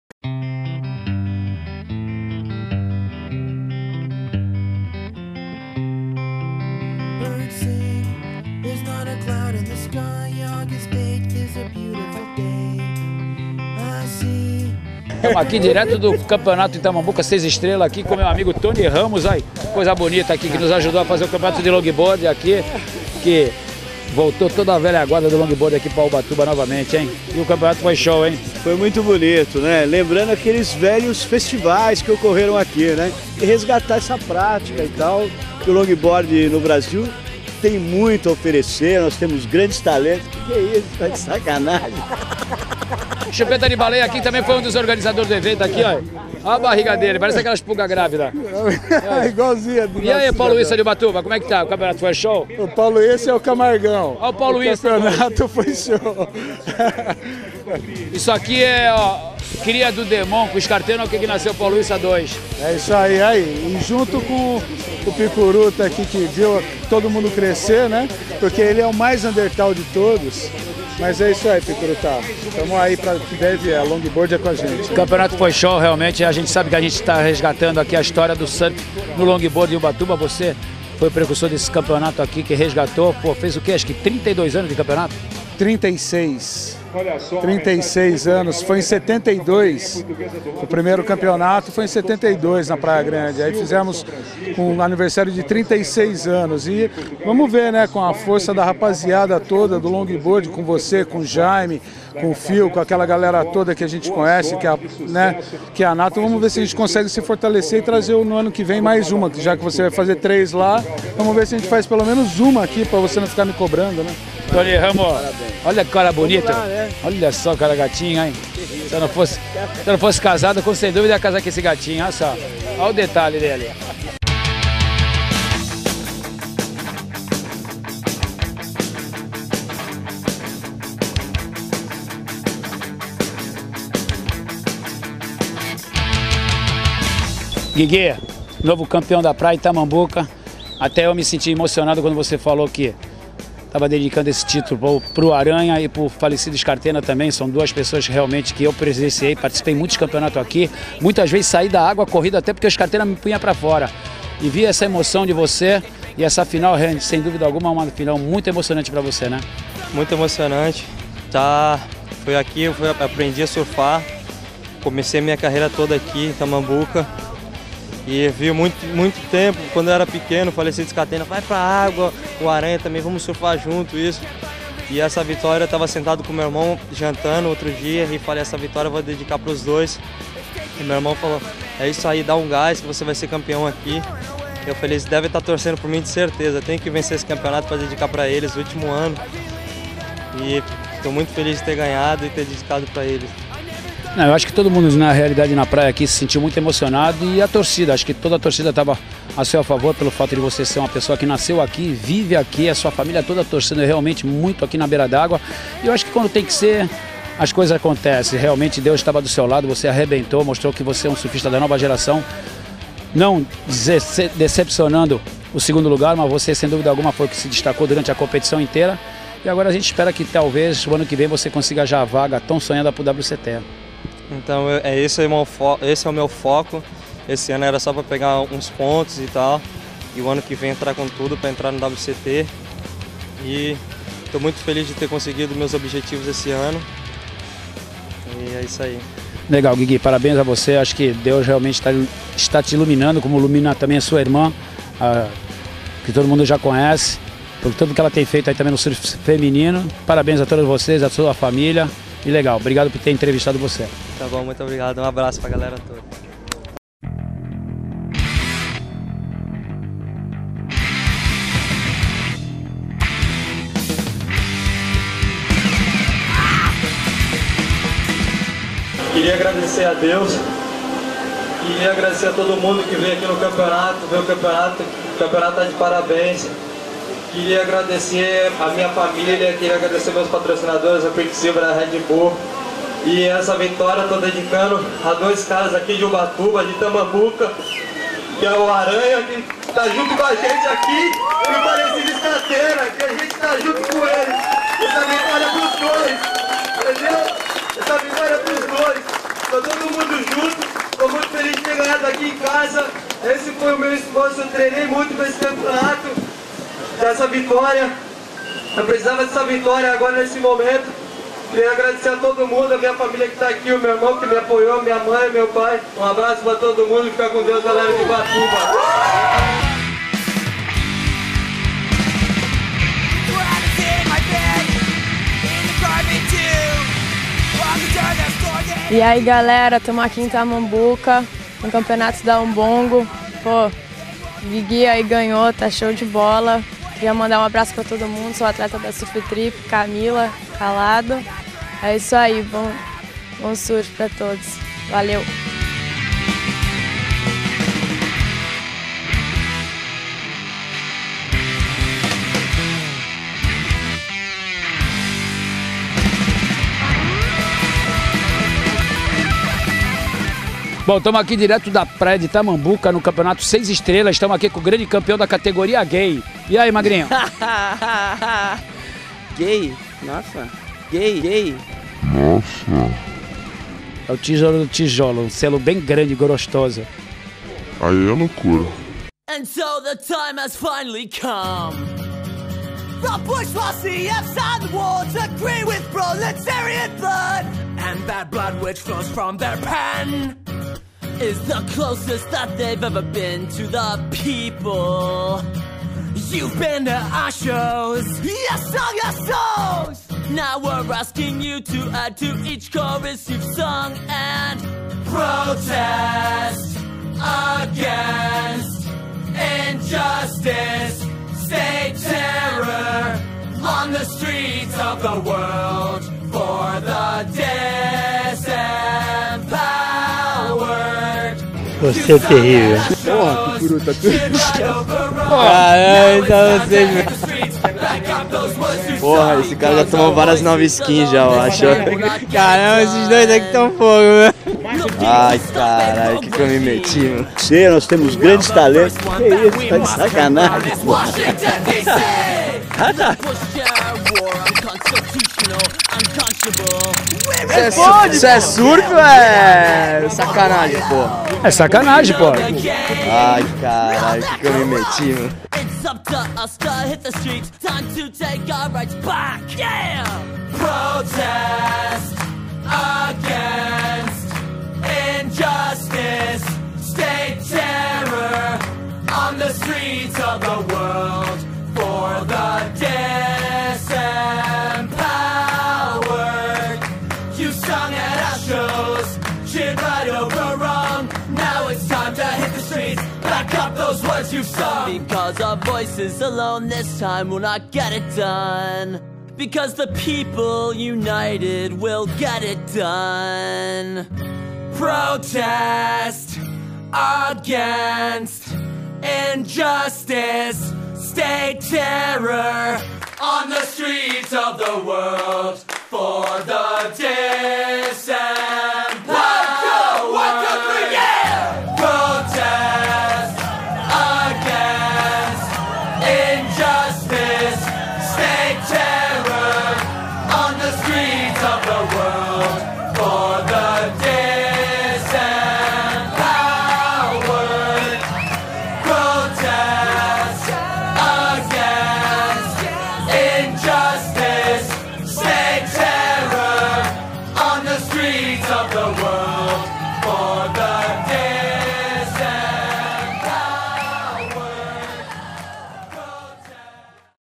Surftrip no WQS em Itamambuca parte 04
Confira a zona que a galera da surftrip com seu repórter e lenda do surf picuruta salazar fez na etapa do WQS, praia de itamambuca! O Picuruta é terrível!